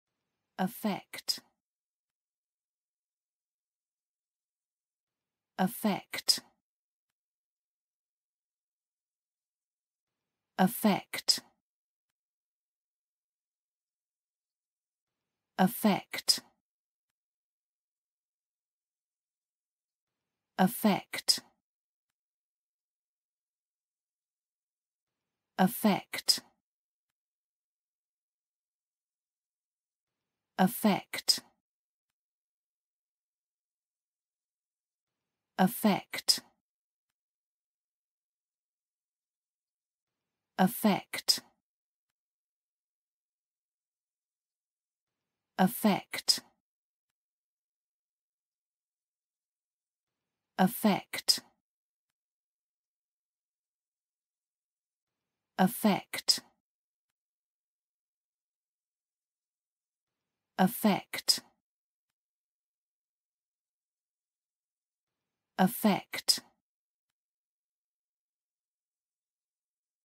affect: How to pronounce affect in english (correct!).Start with A. Learn from me.
Follow me!Learn from me how to pronounce the word affect in english.This video shows you how to pronounce the word affect in english.

affect

vt. 影响；感染；感动；假装vi. 倾向；喜欢n. 情感；引起感情的因素

/əˈfɛkt/

(affecting,affected,affects)

1.   V-T   及物动词

If something affects a person or thing, it influences them or causes them to change in some way. (某事物) 影响 (某人或物)

例： Nicotine adversely affects the functioning of the heart and arteries. 尼古丁对心脏和动脉功能有负面影响。

2.   V-T   及物动词

If a disease affects someone, it causes them to become ill. (疾病) 困扰 (某人)

例： Arthritis is a crippling disease which affects people all over the world. 关节炎是一种严重影响健康、困扰全人类的疾病。

3.   V-T   及物动词

If something or someone affects you, they make you feel a strong emotion, especially sadness or pity. (某事物或人) 影响 (某人)

例： If Jim had been more independent, the divorce would not have affected him as deeply. 如果吉姆曾经更独立一些，离婚就不会对他有这么深的影响了。

4.   V-T   及物动词

to put on an appearance or show of; make a pretence of 假装

例： to affect ignorance